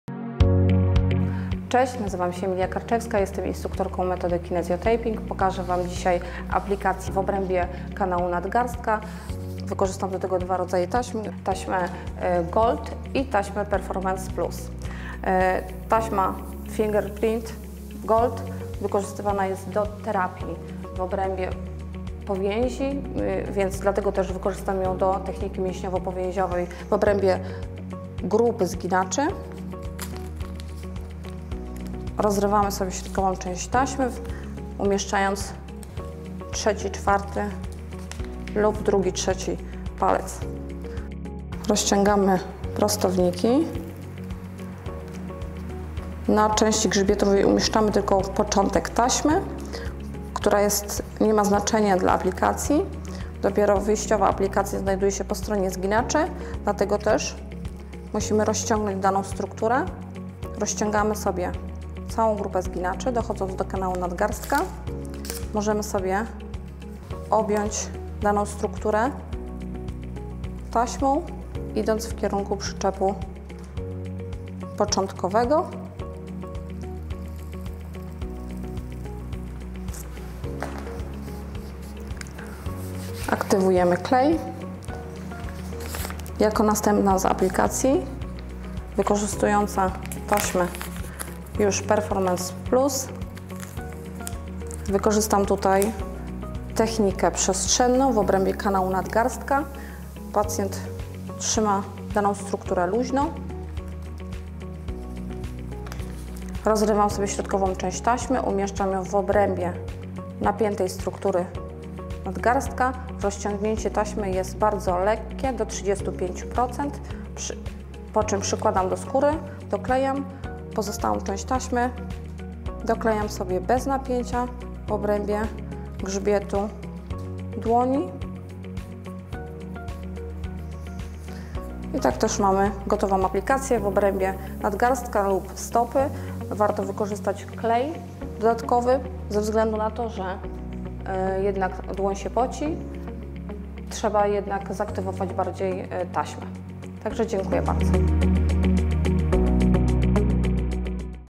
KinesioTaping- ból nadgarstka, zespół cieśni nadgarstka, aplikacja na nadgarstek
mgr Emilia Karczewska Międzynarodowa Instruktorka Metody KinesioTaping prezentuje aplikacje na dolegliwości występujące w obrębie kanału nadgarstka.